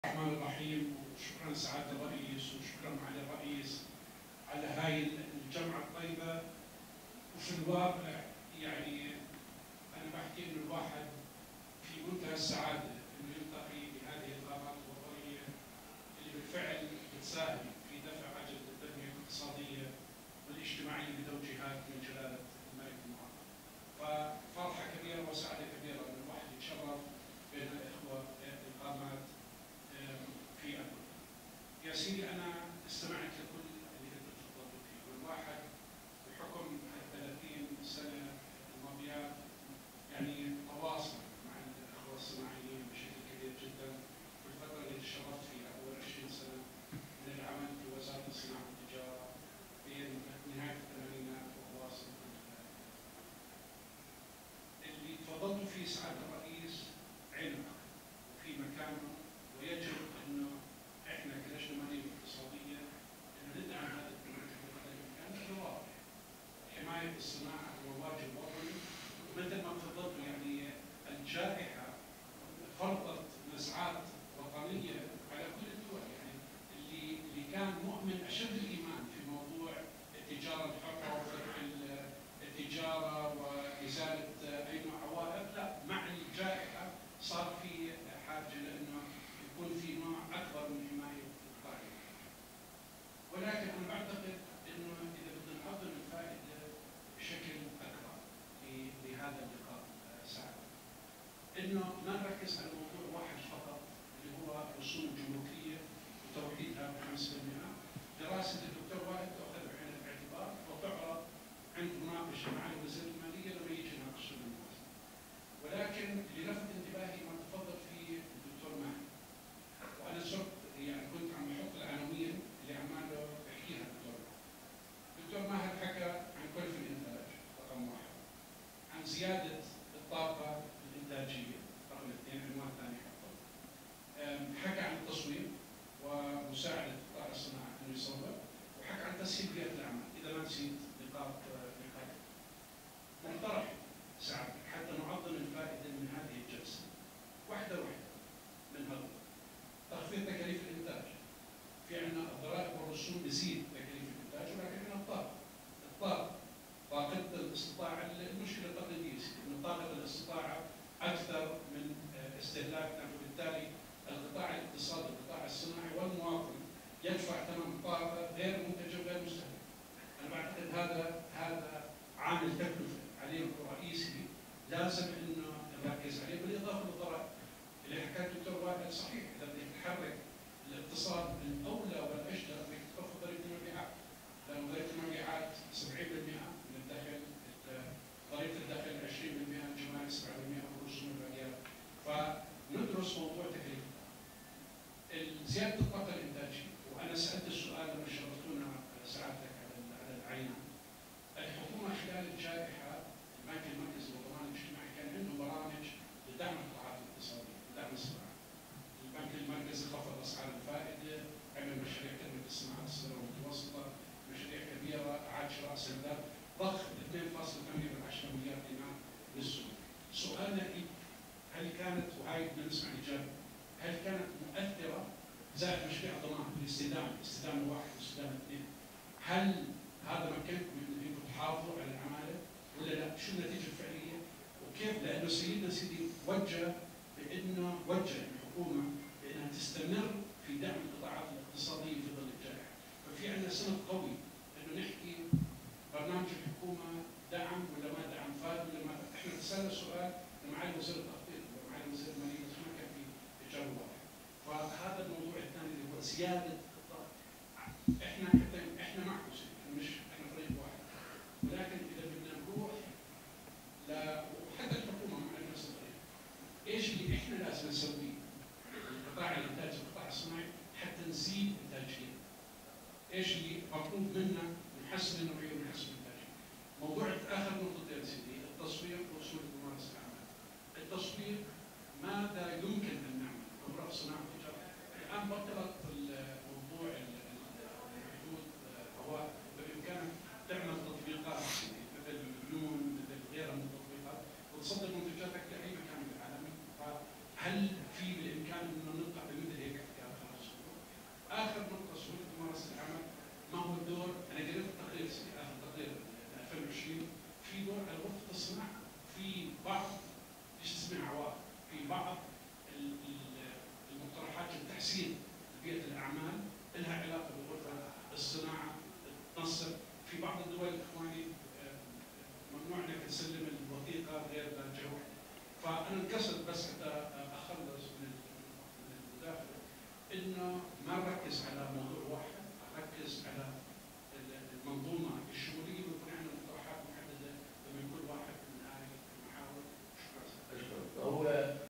لقاء اللجنة الاقتصادية بمجلس الاعيان الاردني باعضاء غرفة صناعة عمان _مداخلات
لقاء اللجنة الاقتصادية بمجلس الاعيان الاردني باعضاء غرفة صناعة عمان